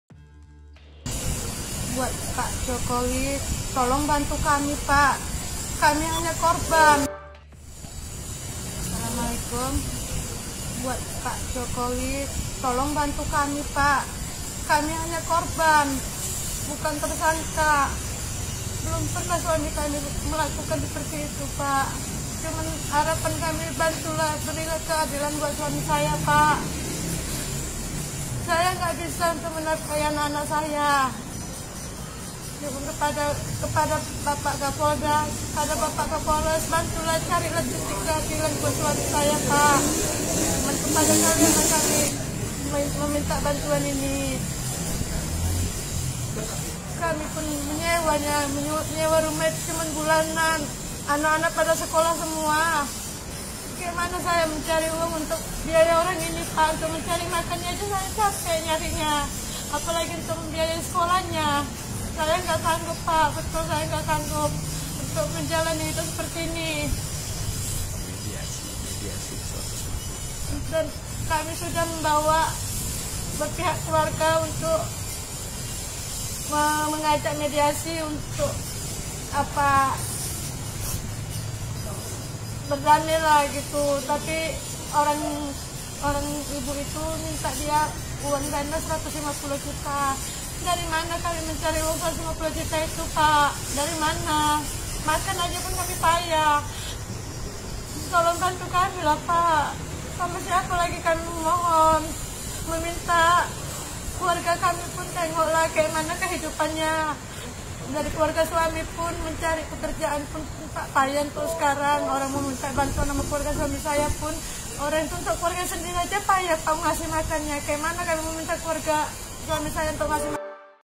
Korban Pengeroyokan Jadi Tersangka, Istri Pengeroyok Minta Tolong Jokowi
TRIBUNJATIM.COM -

Korban Pengeroyokan dilaporkan balik oleh pengeroyoknya ke Polsek Percut Sei Tuan

Hampir bersamaan dengan viralnya surat penangkapan tersangka untuk korban Gea, seorang wanita yang mengaku sebagai istri dari preman Beny

Wanita ini menangis, memohon pertolongan pada Kapolda Hingga Presiden Jokowi

Dia membantah suaminya telah melakukan pengeroyokan, dan menuding korban Gea memulai pertikaian lebih dulu

Kala diajak bicara secara kekeluargaan, keluarga Korban Gea justru diklaim minta uang damai sampai Rp 150 Juta

hingga berita ini diunggah, tribunnews masih menggali info lebih lanjut terkait hal ini

Sumber: Kompas - Tribunnews
Sumber Video:
- Facebook Muh Reyhan Nizam
- Facebook orangdanvlog
- TribunMedan